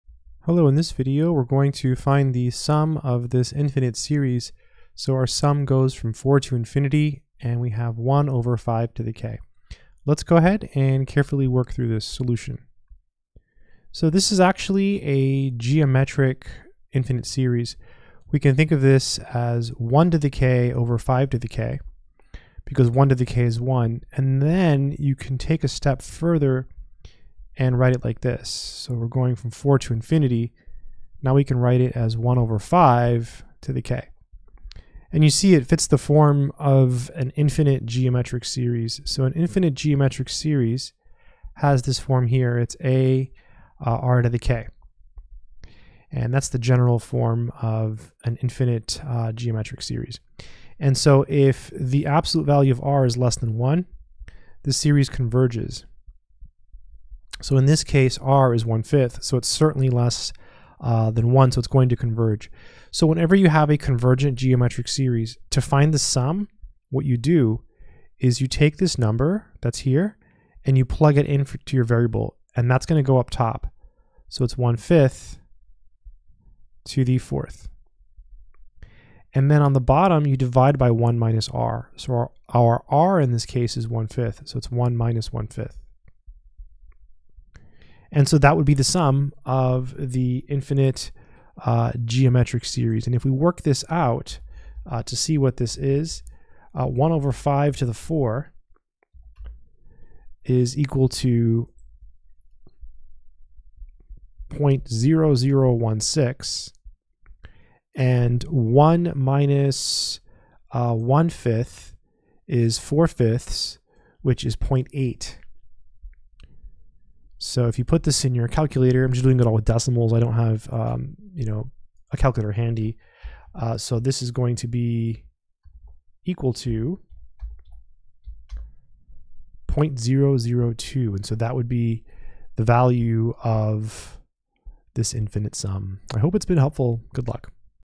Sum of the Infinite Geometric Series SUM(1/5^k)
We find the sum of the infinite geometric series sum(1/5^k). I hope this helps someone who is learning calculus.